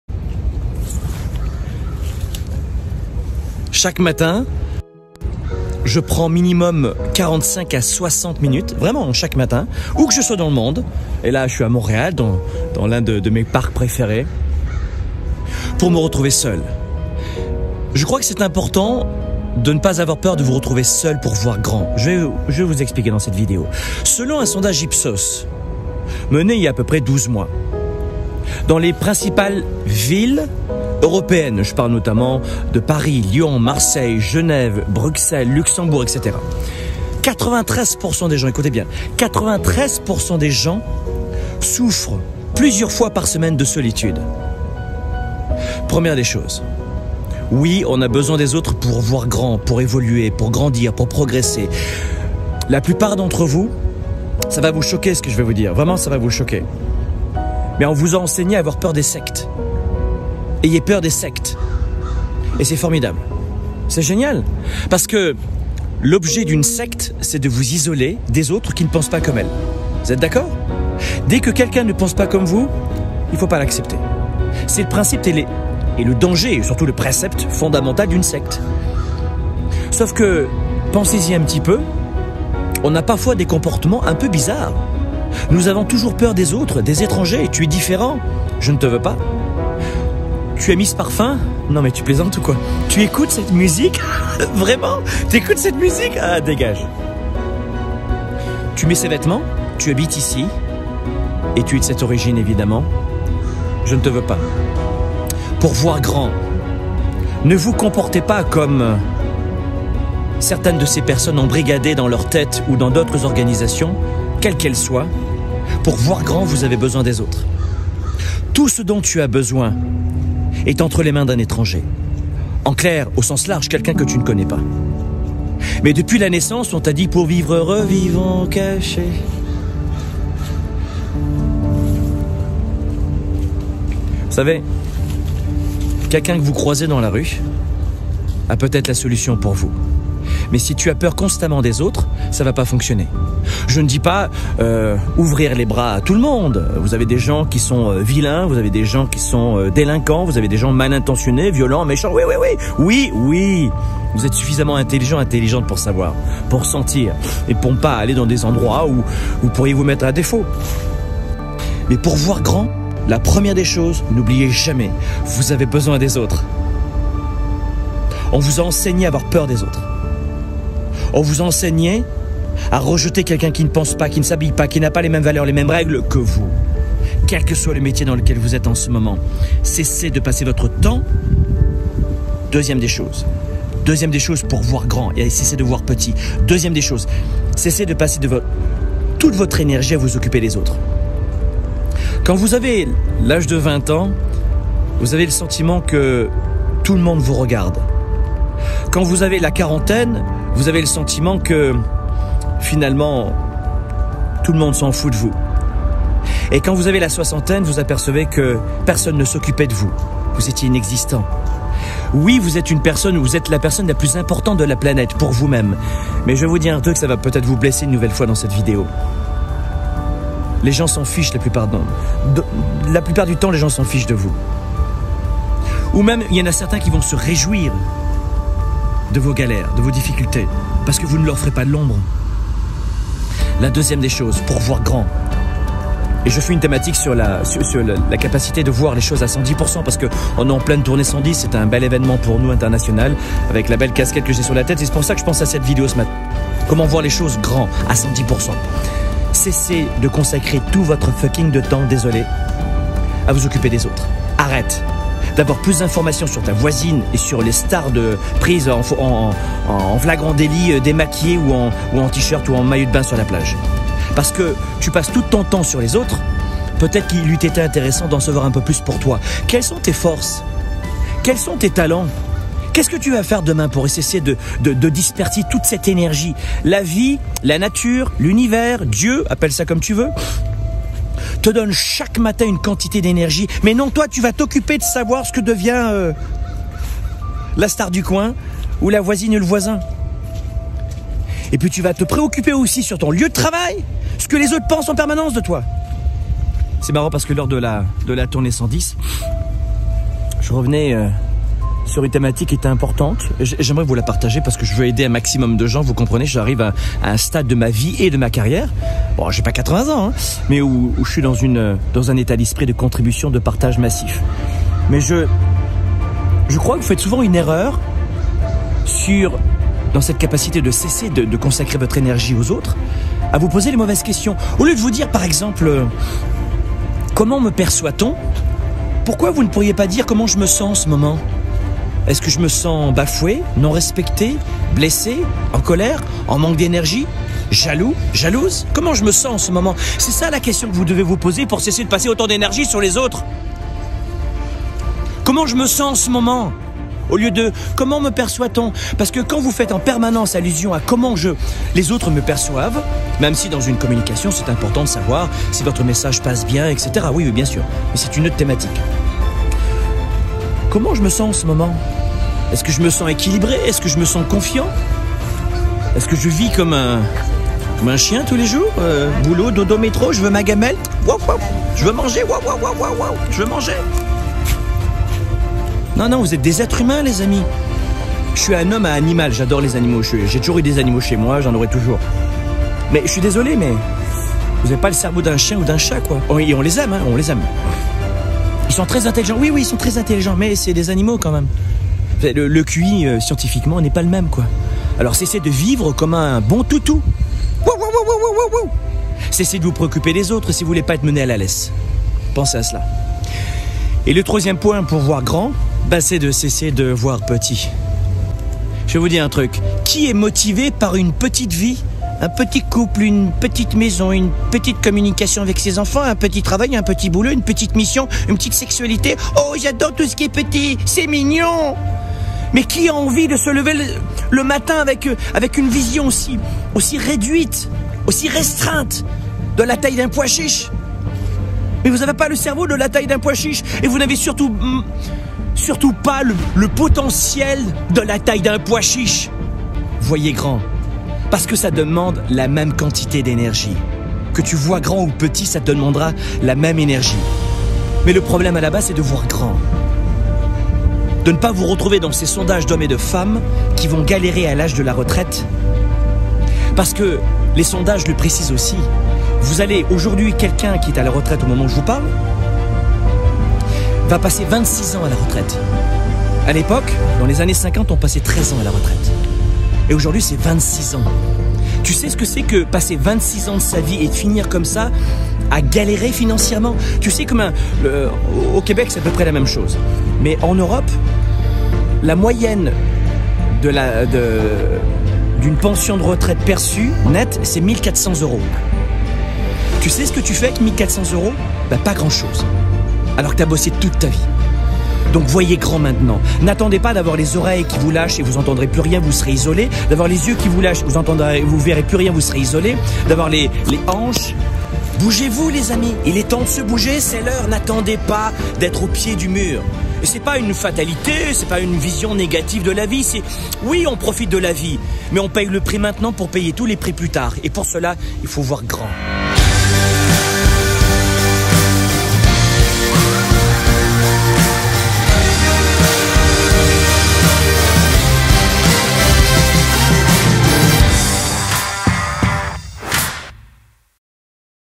Refusez de vivre dans une grande "SECTE" I Franck Nicolas
Sans y prêter garde nous avons été éduqué dans la peur de l'autre et des inconnus.
Nous agissons tel des adeptes d'une grande secte qui rejette tout et tout le monde.
La philosophie de l'Effet 110, enseigné à La Tournée 110,  est justement aux antipodes celui d'aller vers les autres et de voir grand. Voilà deux valeurs essentielles pour vivre votre vie à 110% et ne plus jamais la regretter.